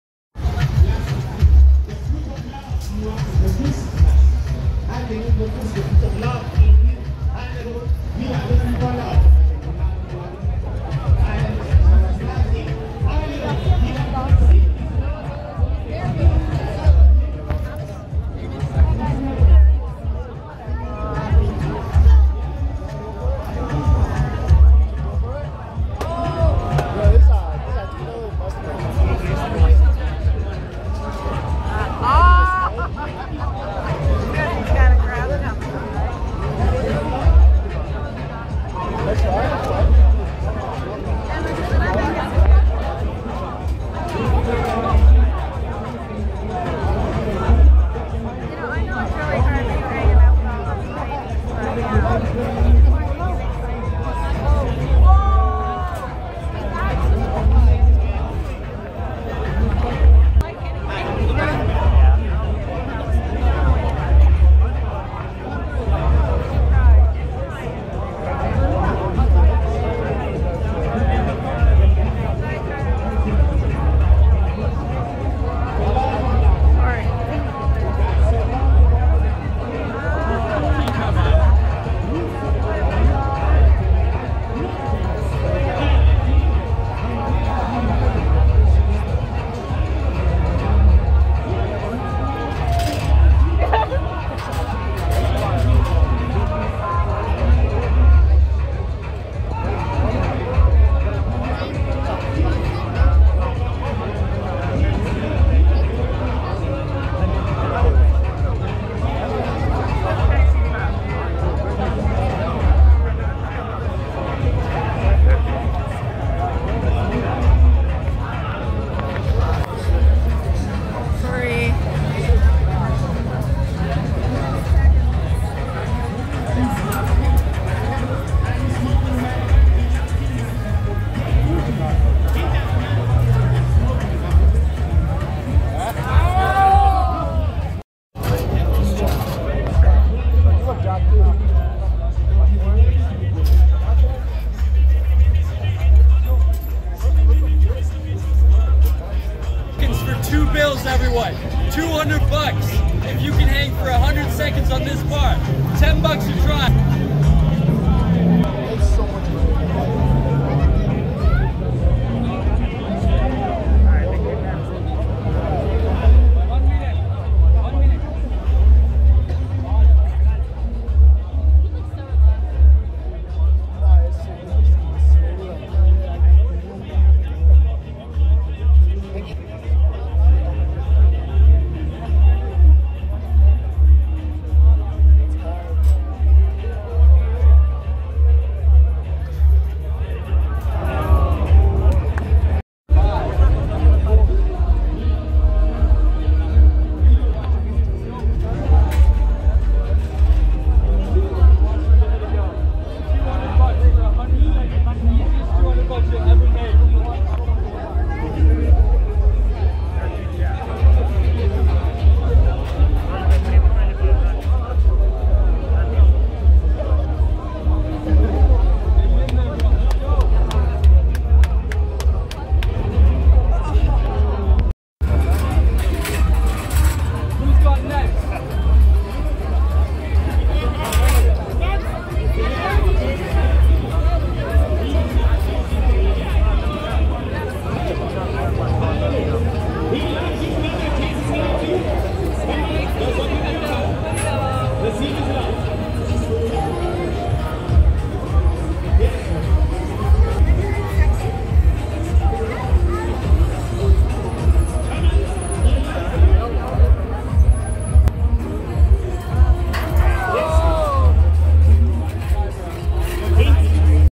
Hanging Challenge in Yonge-Dundas Square, TO. You could win $200!
Could you win this? Hang for 100 seconds, win $200. Pay $10 to try.